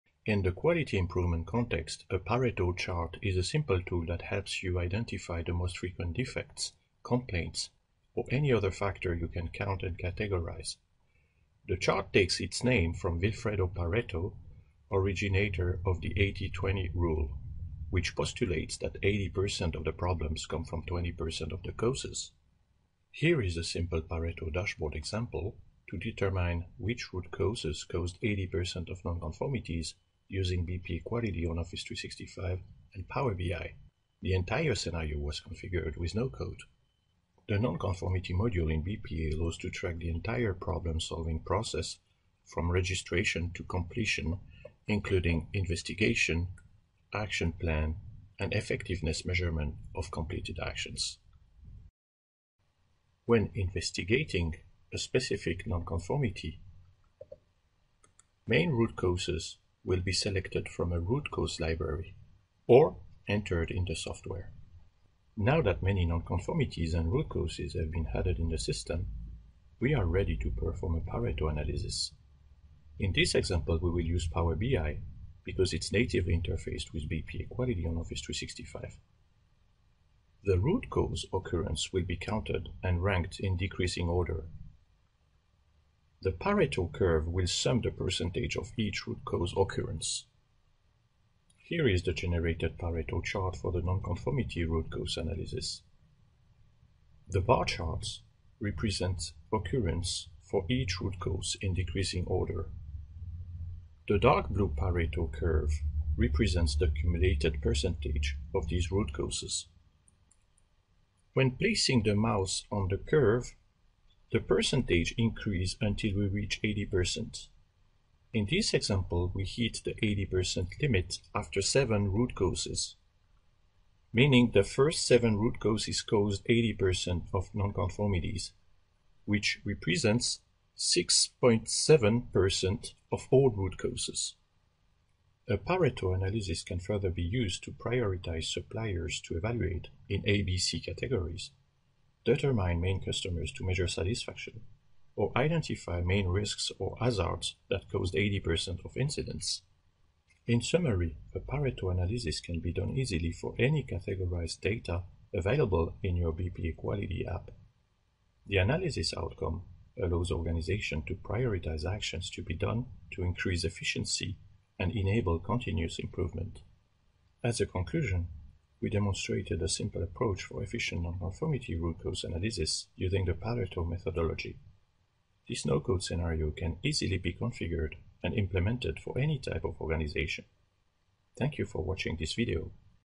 Pareto Analysis with BPA Quality and Office 365/SharePoint
Pareto dashboard example to determine which root causes provoked 80% of nonconformities using BPA Quality on Office 365 and Power BI.